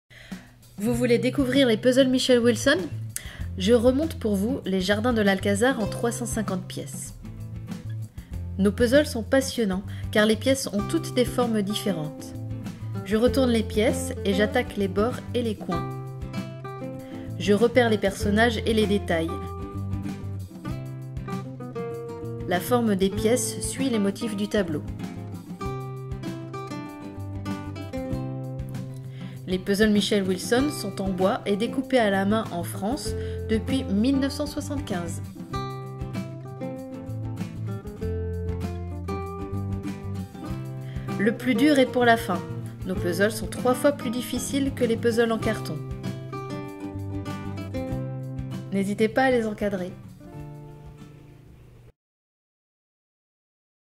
Want to discover the Michèle Wilson Puzzle brand? I'm assembling a wooden puzzle for you!
Vidéo en français et sous-titre en anglais
Want to discover the Michèle Wilson Puzzle brand? For that, I am assembling the "Gardens of the Alcazar" in 350 pieces. Our jigsaw puzzles are exciting because each piece is different.
I flip the pieces over and start assembling the edges and corners first.
I spot the figures and the details. The shape of the pieces follows the details of the painting. Michèle Wilson puzzles are made from wood and cut by hand in France since 1975. The hardest part is at the end. Our puzzles are 3 times more difficult than cardboard puzzles. Feel free to frame them!